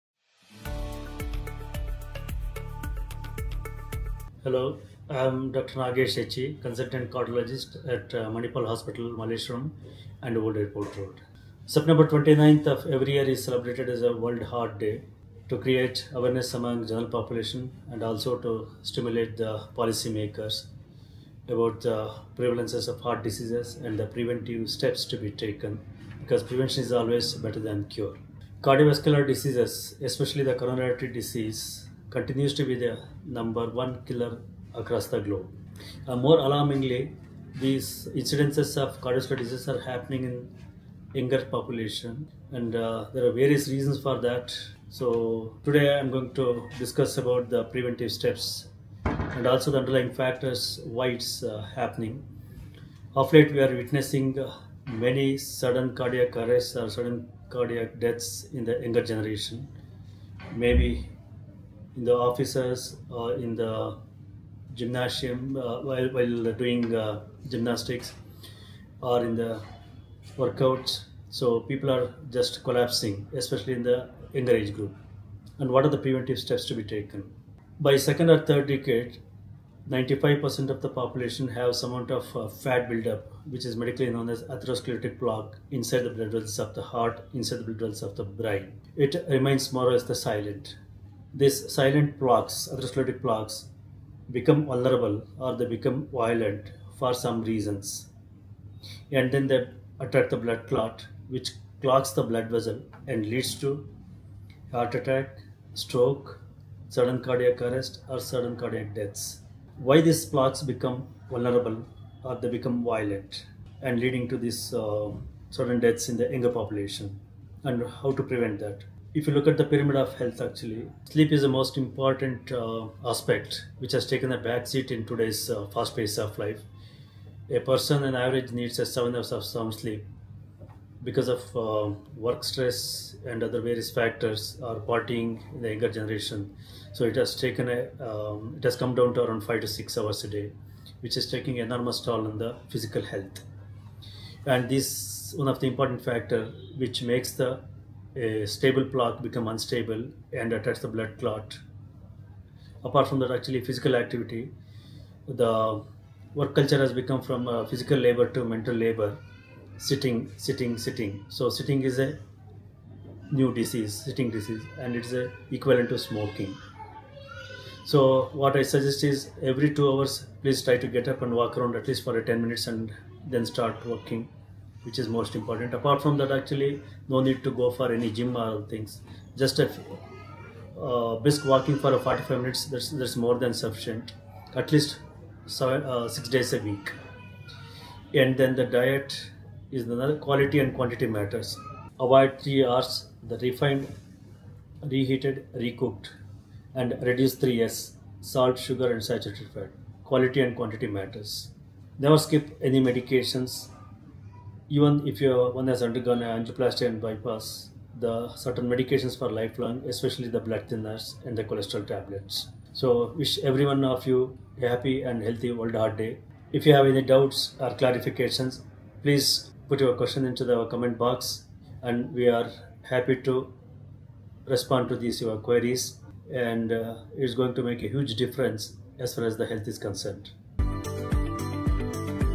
Preventive Measures for Heart Health | Dr. Nagesh H E | Manipal Hospital Malleshwaram
In honor of World Heart Day, observed annually on 29th September, Dr. Nagesh H E, Consultant – Cardiology, Manipal Hospital Malleshwaram, emphasizes the importance of preventive measures for maintaining heart health.

Cardiovascular disease, especially Coronary Artery Disease, is one of the leading causes of death worldwide. Lifestyle factors such as obesity, poor diet, lack of physical activity, smoking, and alcohol consumption significantly increase the risk, even among younger individuals. Inadequate sleep also plays a critical role in heart health.

Dr. Nagesh H E advises regular physical activity, such as brisk walking for 45 minutes, 5-6 days a week, as a key preventive measure. He also recommends avoiding refined, reheated, and recooked foods, while limiting salt, sugar, and saturated fats to support a healthier heart.